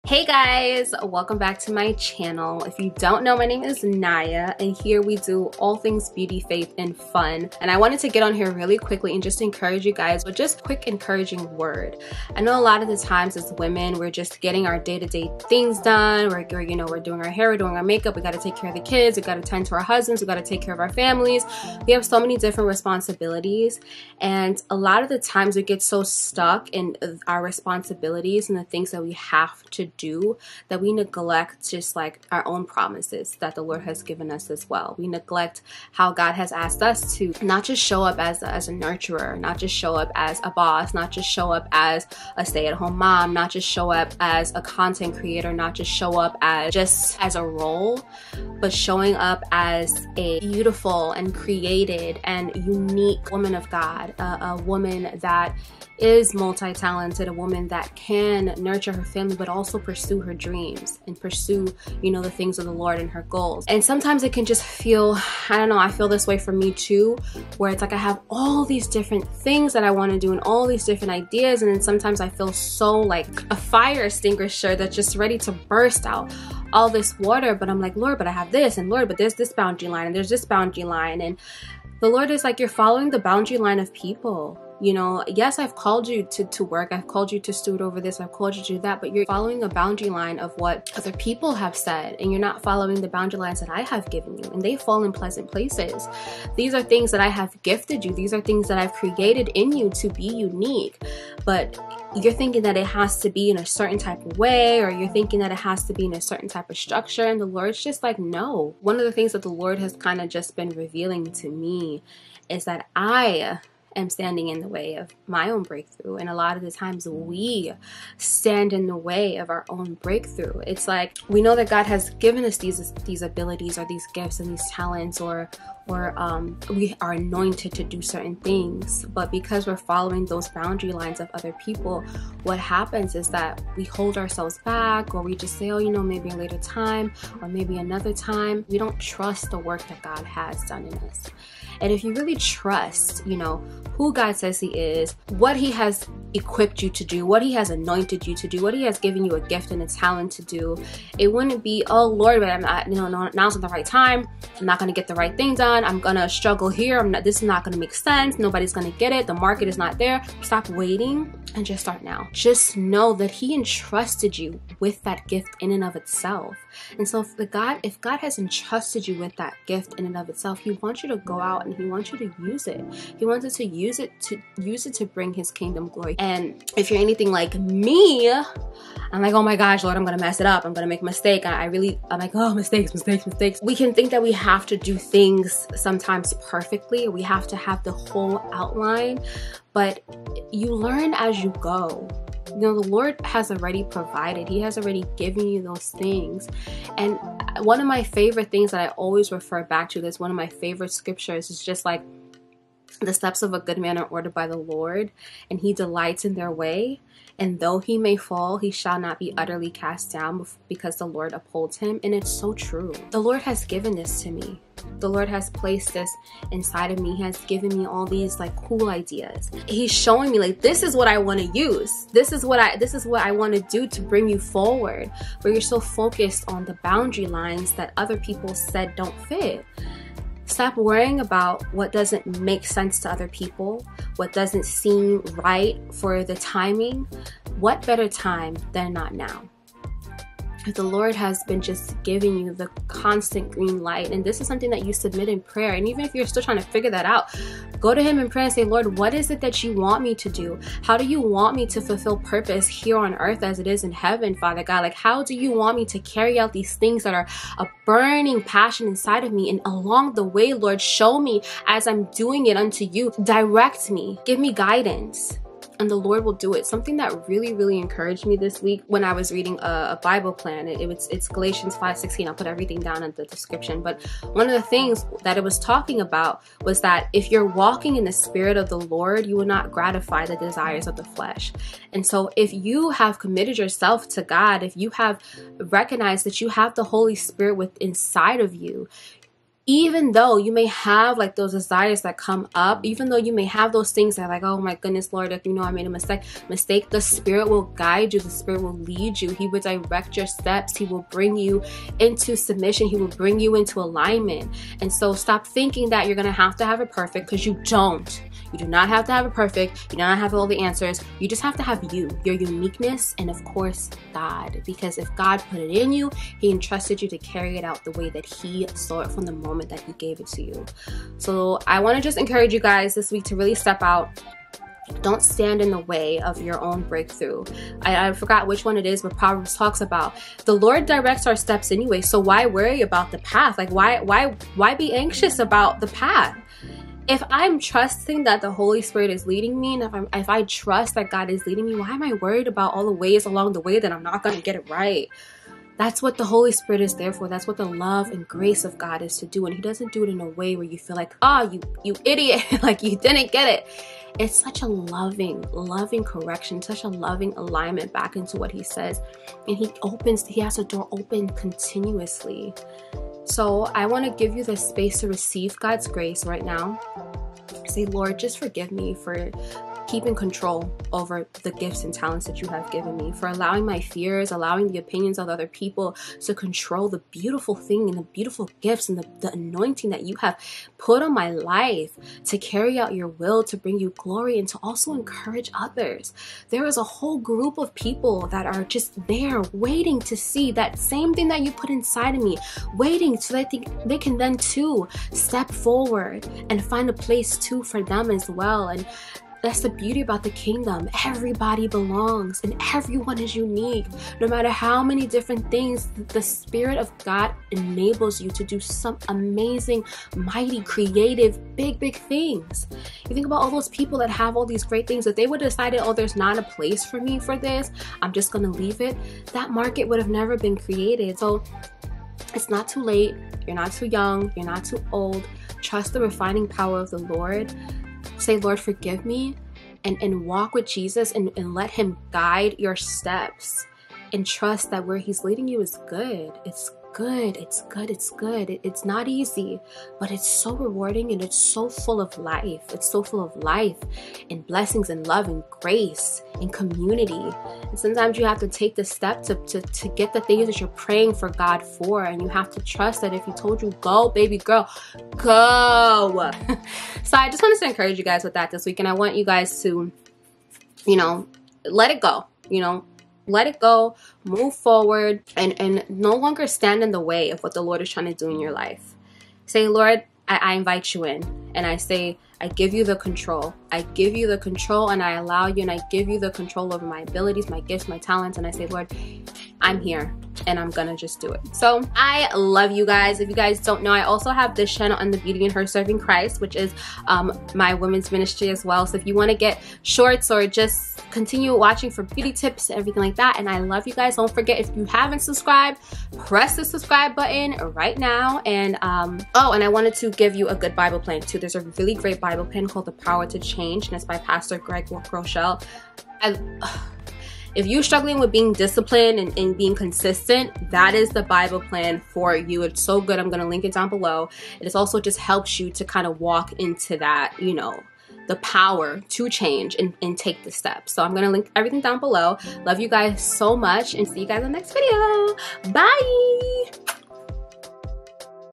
Pushing Past Boundaries To Reach BreakThrough 🤎 | BIHSC
Hi Lovelies!

Welcome back to the channel! I hope today's video encourages you to step out in faith and TRUST IN the LORD.

Scriptures:
- Psalms 37:23-24
- Galatians 5:16
- Proverbs 20:24

Bible Plan - "The Power Change" on the Youversion Bible App

SC: NallyLee_